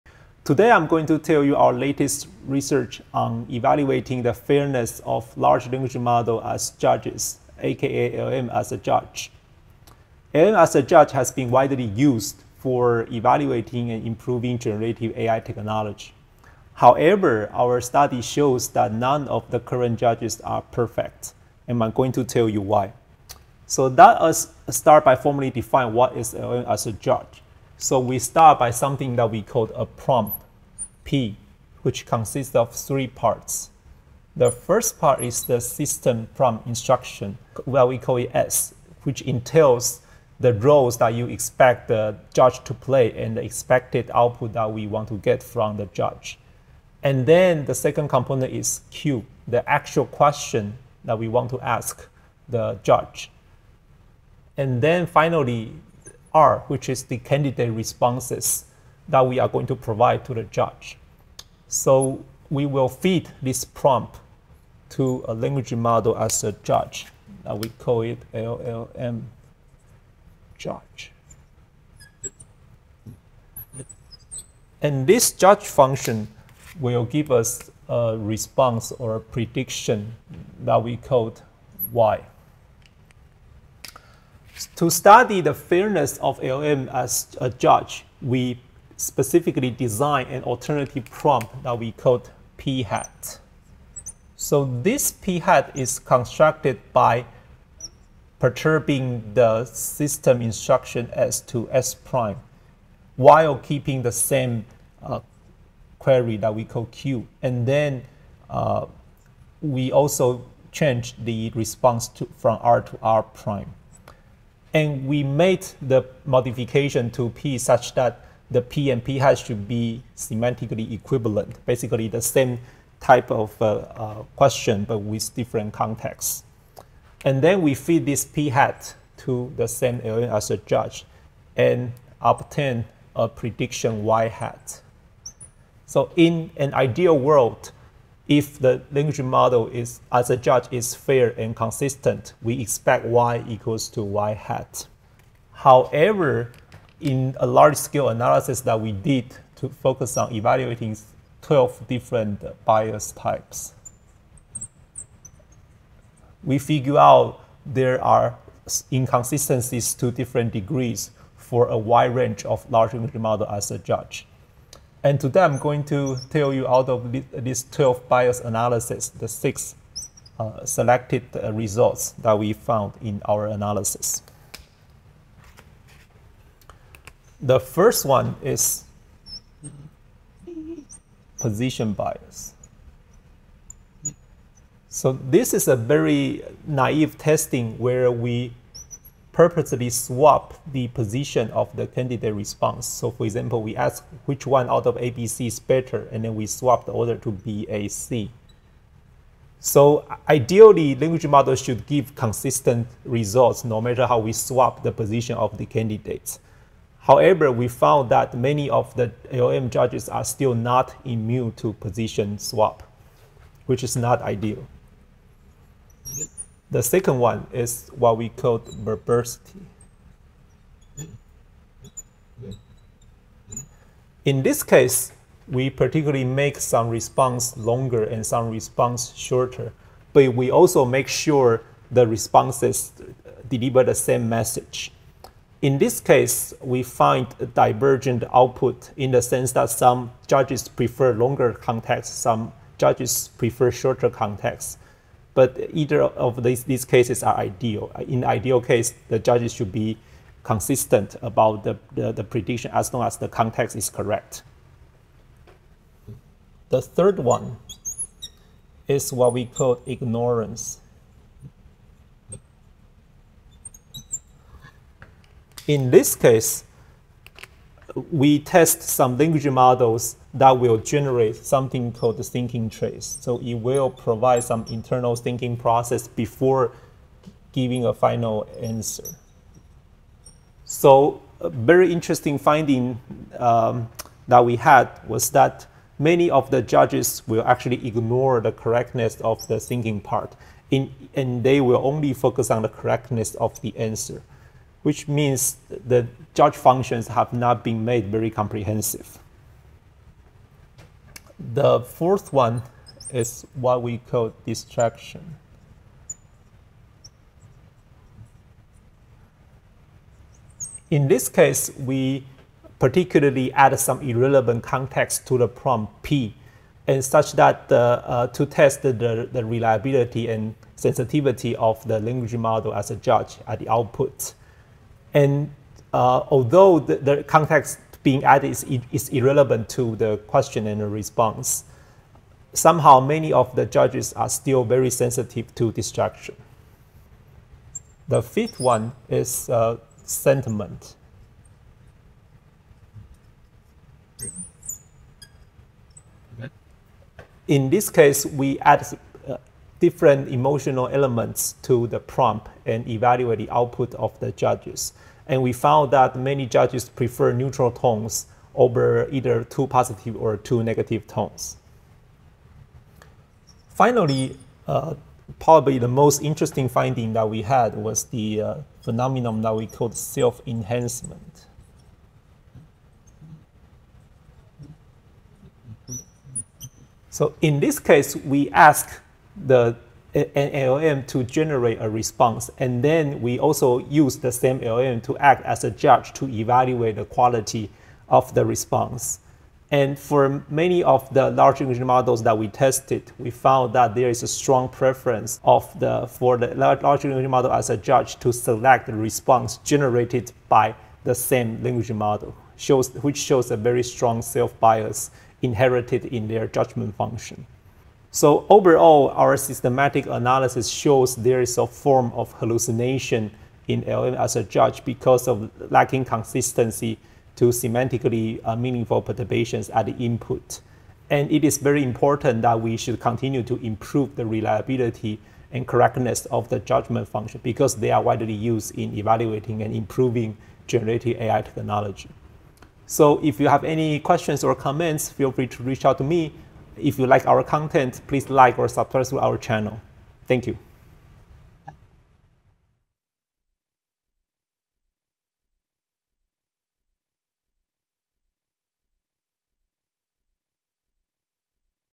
Can You Trust an AI to Judge Fairly? Exploring LLM Biases
Ready to become a certified watsonx AI Assistant Engineer?

Can AI truly judge fairly? 🤔 Pin-Yu Chen explores fairness challenges in Large Language Models (LLMs) as judges, revealing biases like position, verbosity, and self-enhancement. Discover how improving reliability in generative AI impacts ethical decision-making. 🌐

AI news moves fast. Sign up for a monthly newsletter for AI updates from IBM